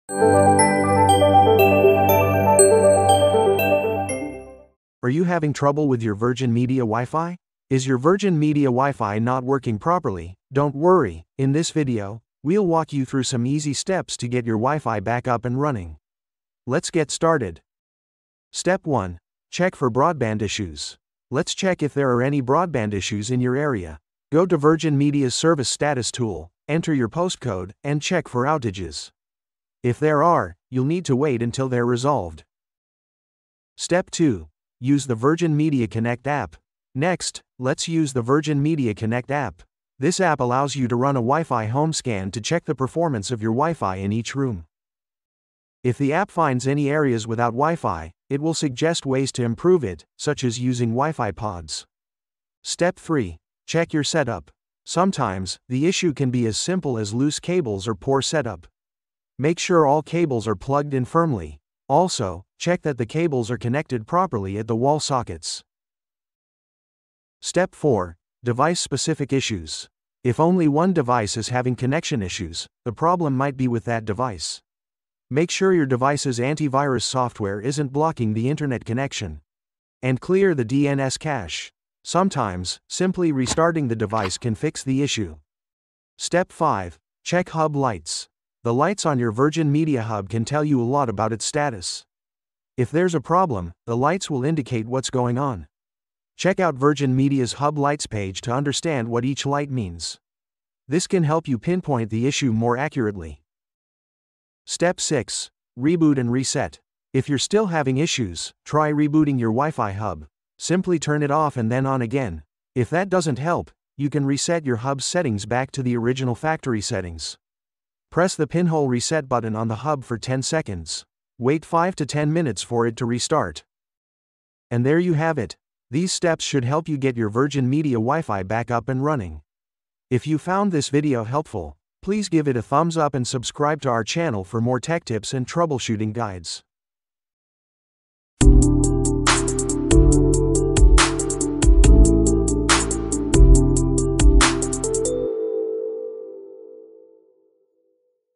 Virgin Media Wifi Not Working? Virgin Media Wifi Not Connecting to The Internet Let's fix it
Is your Virgin Media WiFi not working? And your virgin media wifi not connecting to the internet Don’t worry, we’ve got you covered! In this video, we'll walk you through simple steps to get your WiFi back up and running. Follow these easy troubleshooting tips and get back online in no time.